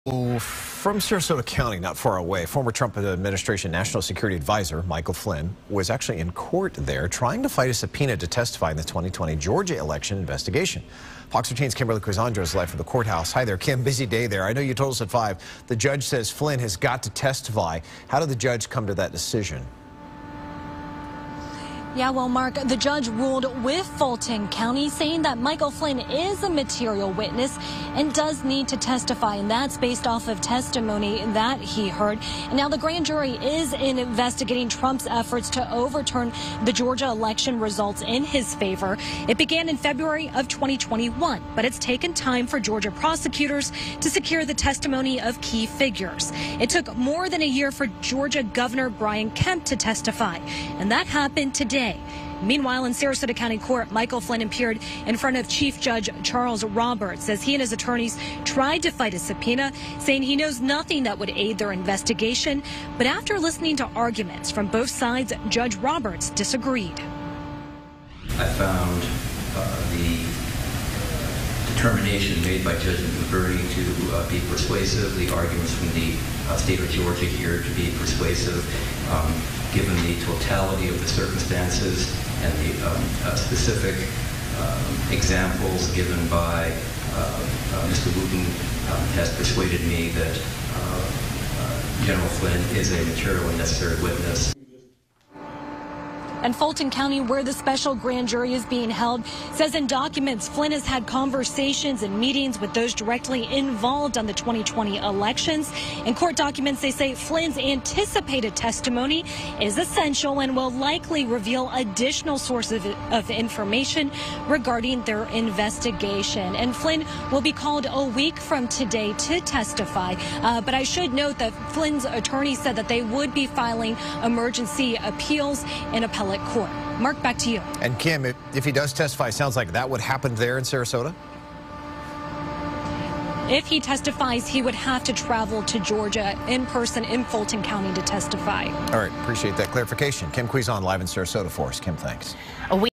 Michael Flynn fights subpoena in Sarasota courtroom
Former Trump administration national security advisor Michael Flynn was in court trying to fight a subpoena to testify in the investigation into former President Trump's alleged efforts to overturn the Georgia election results in 2020.